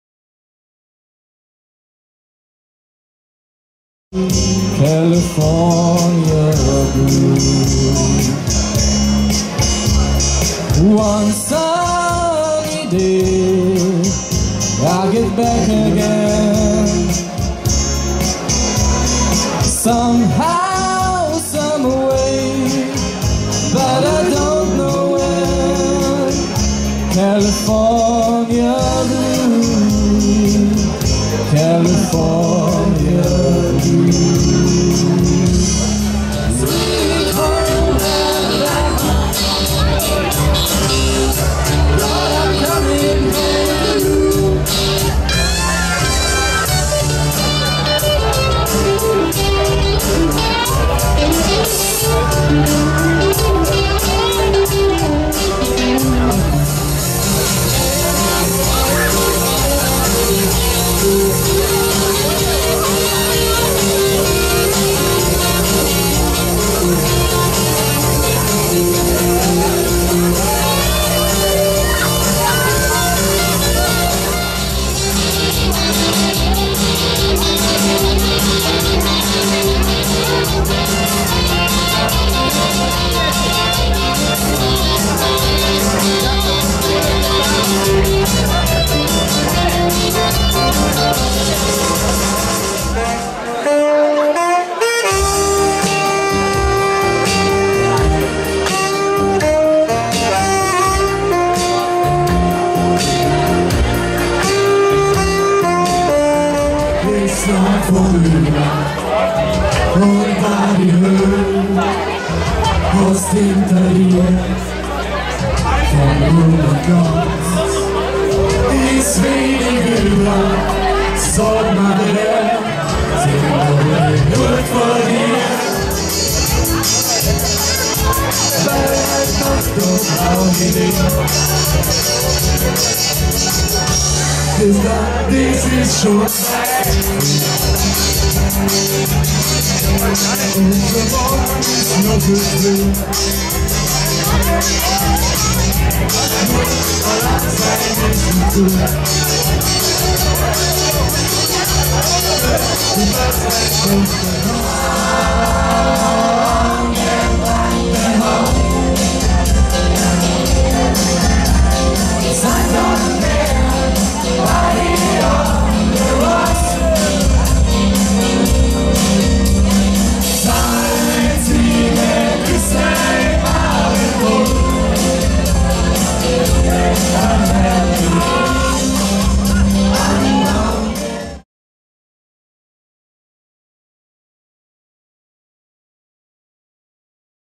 Rosental Express - Holzhackerball 2014
Zusammenschnitt Modernes Programm

Rosental Express (Trio Besetzung) am 18.Jänner beim Gasthof Sereinig im Bodental

0664/1353209